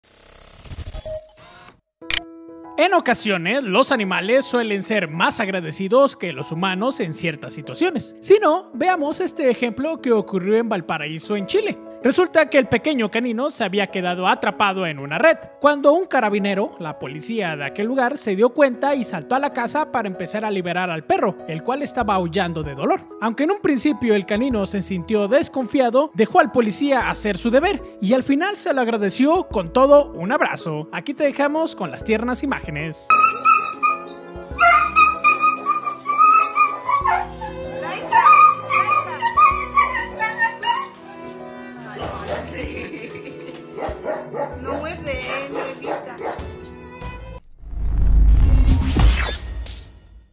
🐶😍Perrito agradece a policía que lo rescató🐶😍
Los animales suelen ser más agradecidos que los humanos en ciertas ocasiones, y para muestra, lo que ocurrió en Chile, cuando un policía liberó a un can de su prisión, este no hizo más que abrazarlo.